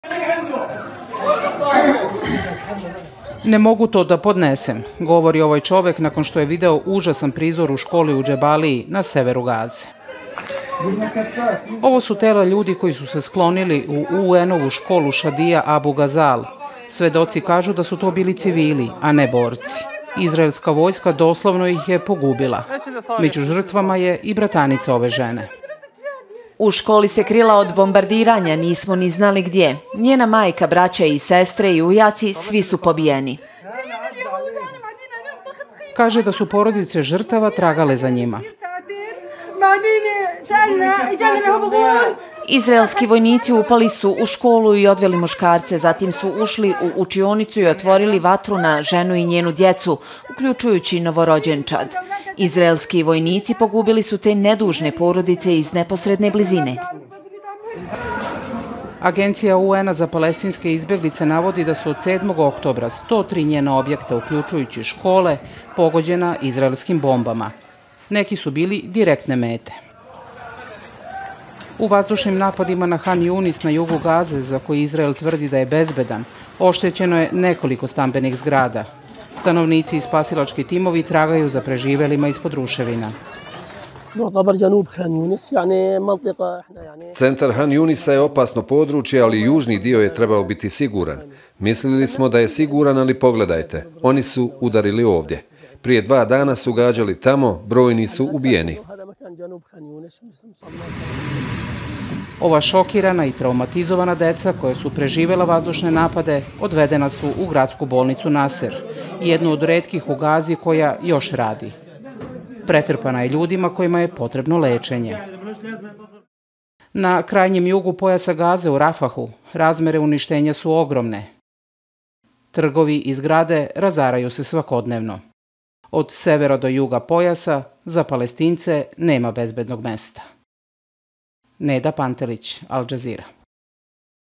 Izraelska vojska u školi pogubila palestinske civile
Al Jazeera je došla do snimaka na kojima se vide tijela ubijenih u školi Shadia Abu Ghazaleh, zapadno od izbjegličkog kampa Jabalija.
Svjedoci kažu da su izraelske snage ubile nekoliko ljudi, uključujući žene, djecu i bebe koji su se skrivali u školi. Navode da su vojnici odveli sve muškarce, a potom otvorili vatru na civile.